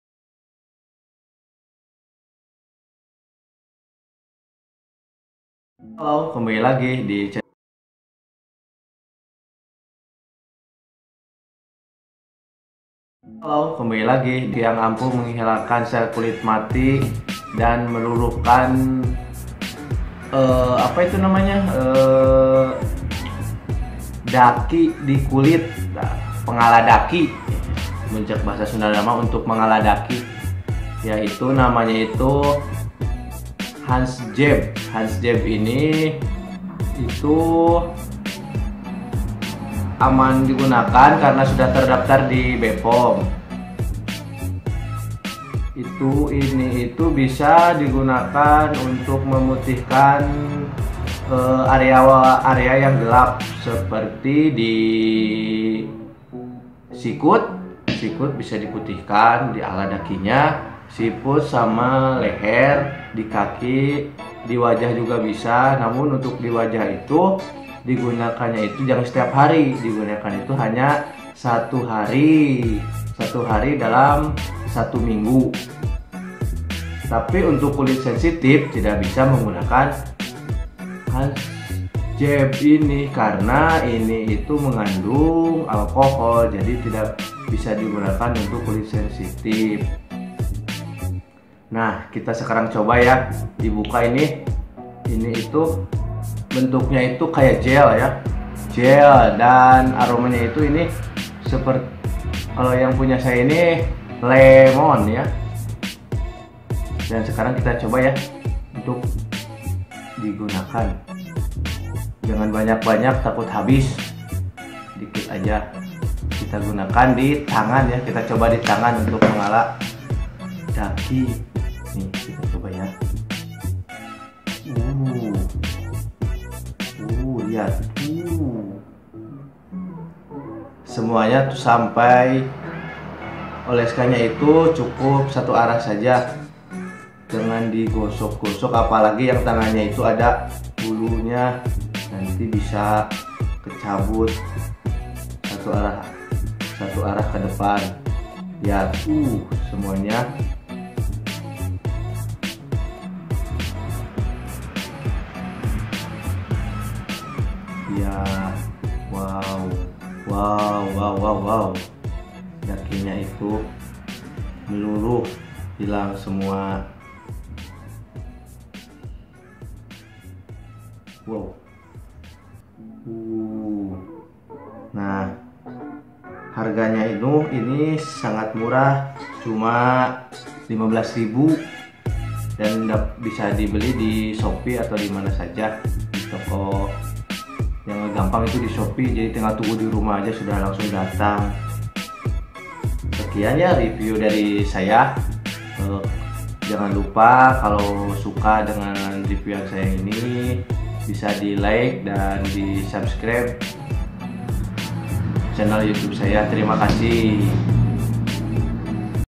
REVIEW HANS JEBB - Penghilang Daki dan pemutih Ketiak
Kali ini aku review produk lokal yaitu Salsa Han's Jebb. Yang merupakan exfoliating untuk badan semacam lululr tapi nggak bakal sakit saat dipakai. Sangat ampuh untuk mengangkat sel kulit mati/daki membandel.

selain itu juga hans jebb dapat memutihkan area gelap seperti ketiak belakang leher sikut dan dengkul.